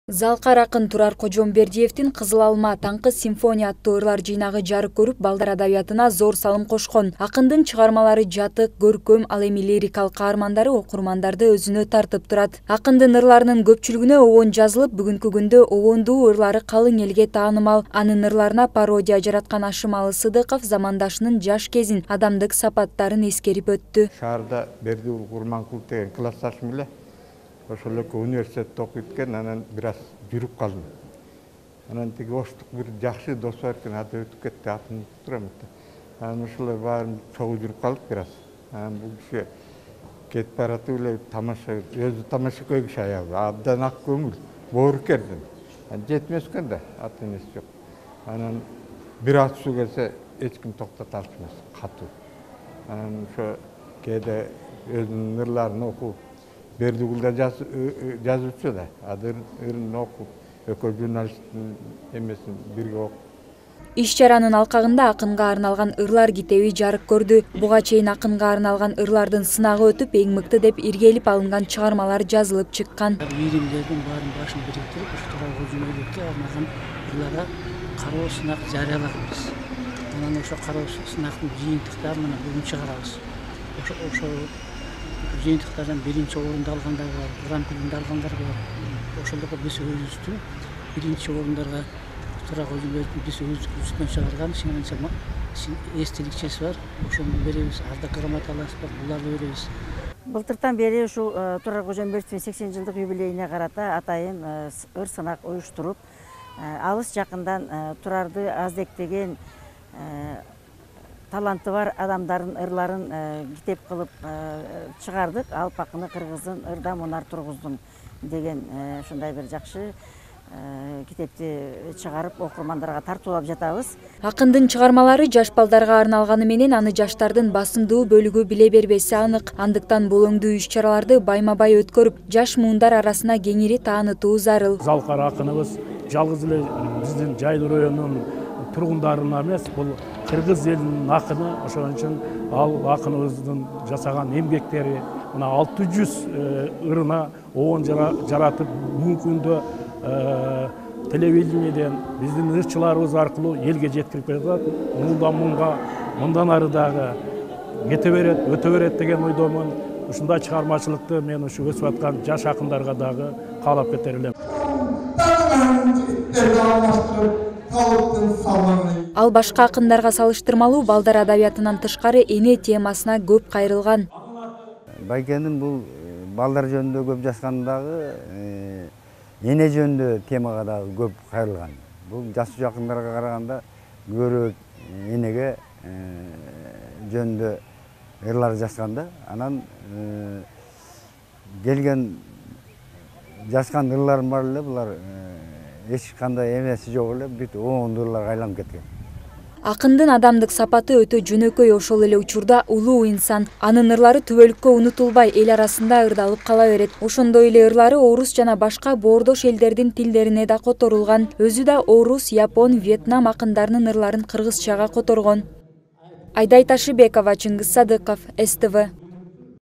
ТУРАР КОЖОМБЕРДИЕВДИН 80 ЖЫЛДЫГЫ
Кыргыз адабиятына өзгөчө орду бар акын- Турар Кожомбердиевдин 80 жылдыгы Кара-Балта шаарынын Ленин атындагы маданият үйүндө белгиленди. Иш-чаранын алкагында акынга арналып жазылган китептин бет ачары өтүп, ага белгилүү ишкерлер, акындын бир туугандары катышып, залкардын улуу чыгармачыл жолун эскеришти.